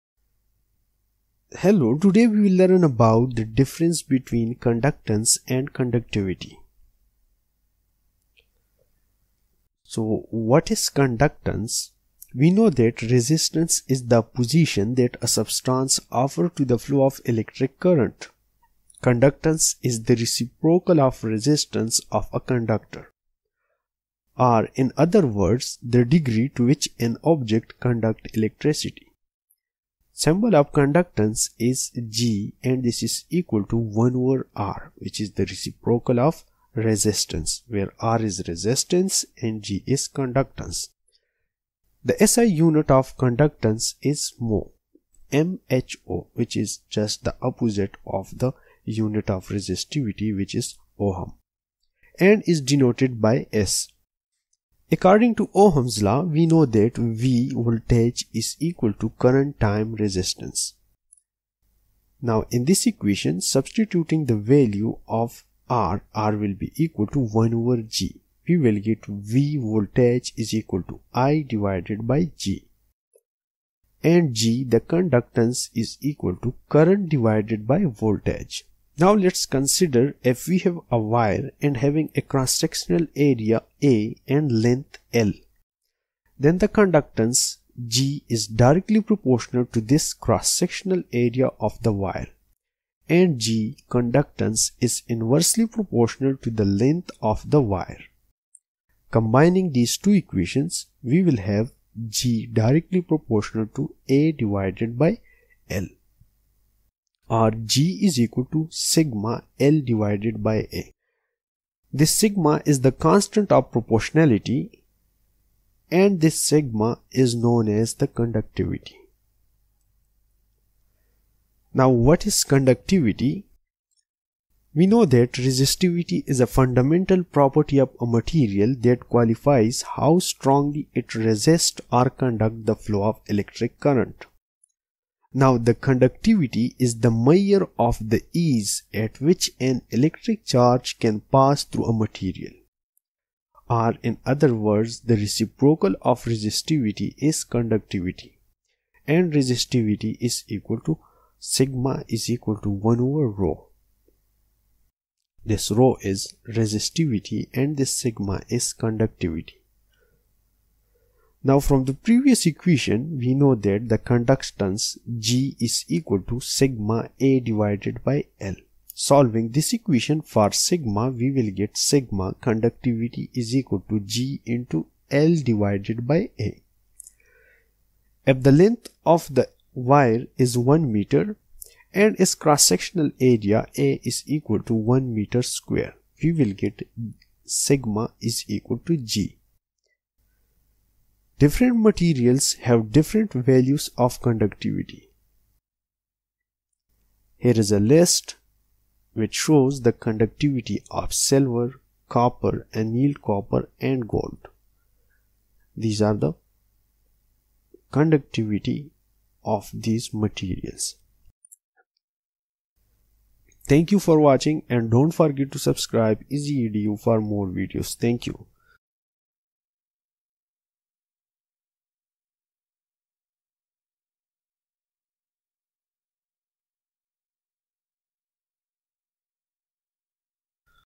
Conductance and Conductivity differences physics
Difference between Conductance, Conductivity, resistance and resistivity
2𝑛𝑑 𝑦𝑒𝑎𝑟 𝑝ℎ𝑦𝑠𝑖𝑐𝑠 𝑐𝑢𝑟𝑟𝑒𝑛𝑡 𝑎𝑛𝑑 𝑒𝑙𝑒𝑐𝑡𝑟𝑖𝑐𝑖𝑡𝑦
Conductance
Resistance is the opposition that a substance offers to the flow of electric current
Conductance is the reciprocal of resistance of a conductor or the degree to which an object conducts electricity.
𝑠𝑦𝑚𝑏𝑜𝑙 𝑜𝑓 𝑐𝑜𝑛𝑑𝑢𝑐𝑡𝑎𝑛𝑐𝑒 𝑖𝑠= 𝑮=𝟏/𝑹
si unit of conductance mho is siemens denoted by 𝑆
According to ohms law V=𝐼𝑅⇒𝑉=𝐼/𝐺=𝐺=𝐼/𝑉
Resistance depending on dimension of the wire
𝐺∝𝐴
𝐺∝1/𝐿
𝐺∝𝐴/𝐿=𝜎 𝐿/𝐴  sigma 𝜎=𝑐𝑜𝑛𝑑𝑢𝑐𝑡𝑖𝑣𝑖𝑡𝑦
Conductivity
Resistivity, is a fundamental property of a material that quantifies how strongly it resists or conducts the flow of electric current.
Conductivity is the measure of the ease at which an electric charge can pass through a material.
Reciprocal of resistivity is conductivity   𝜎=1/𝜌
𝐺∝𝐴/𝐿=𝜎 𝐴/𝐿
      𝜎=𝐺 𝐿/𝐴
If 𝐴=1𝑚^2  𝑎𝑛𝑑 𝐿=1𝑚
𝜎=𝐺
Different materials have
different values of conductivity